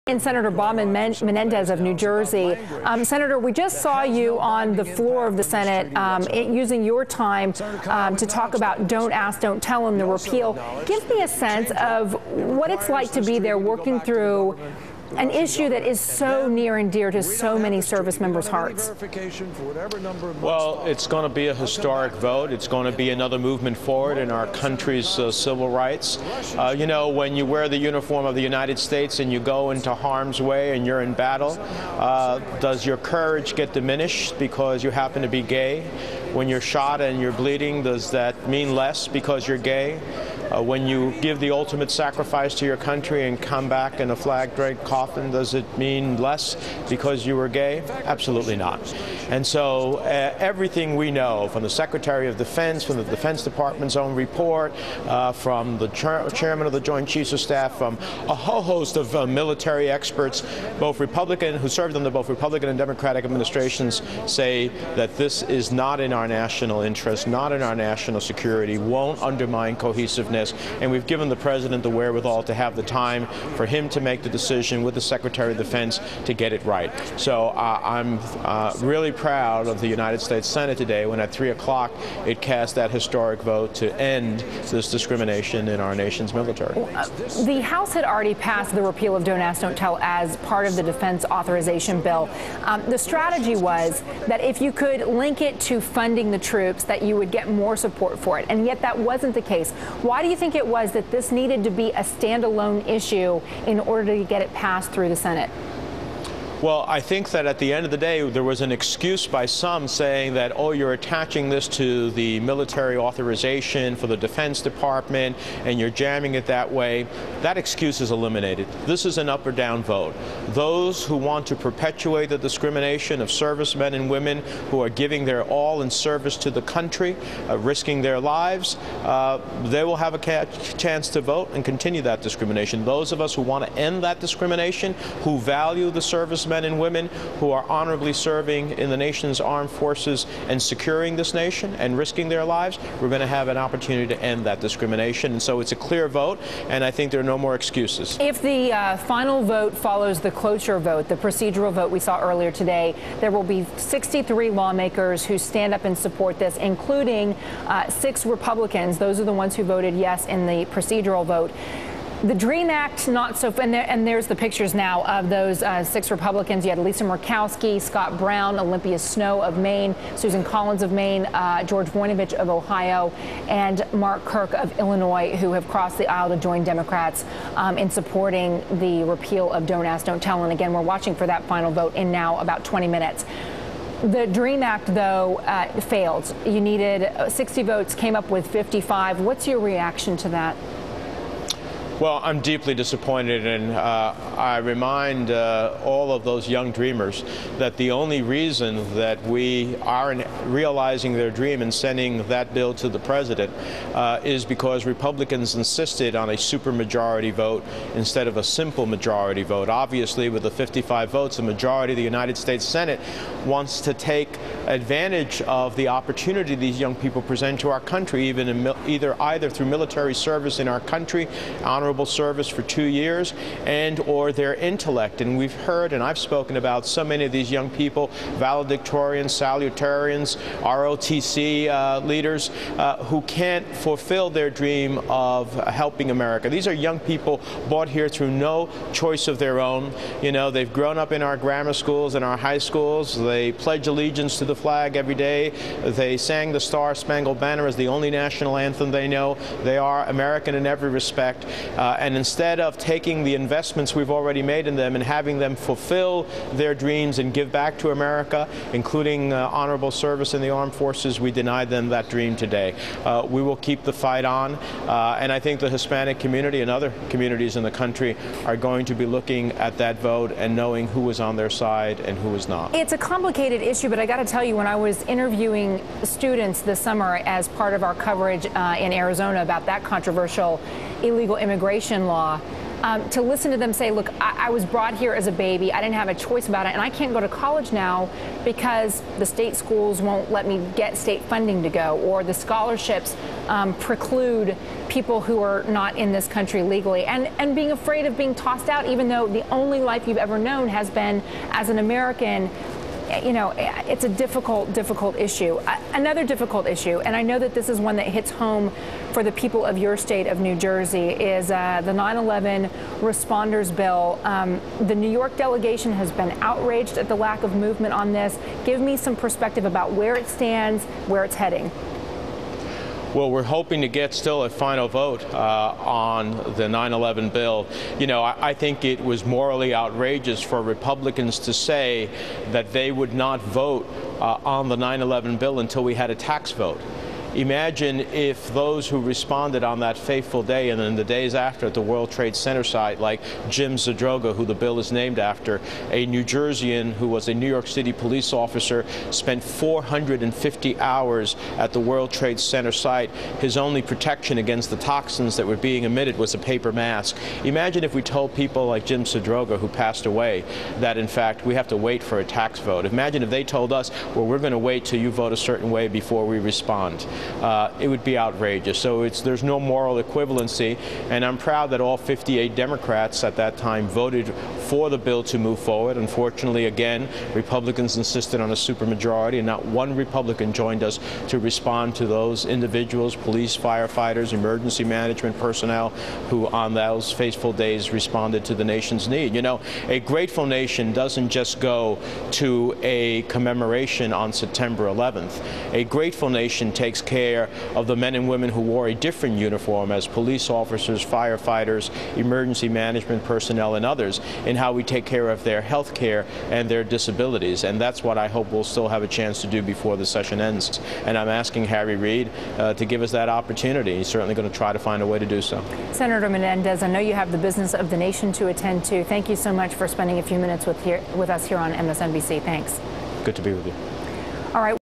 Senator Menendez Advocates For Repealing the Don't Ask Don't Tell Policy
Senator Robert Menendez speaks with MSNBC on the need to immediately repeal the Don't Ask Don't Tell Policy.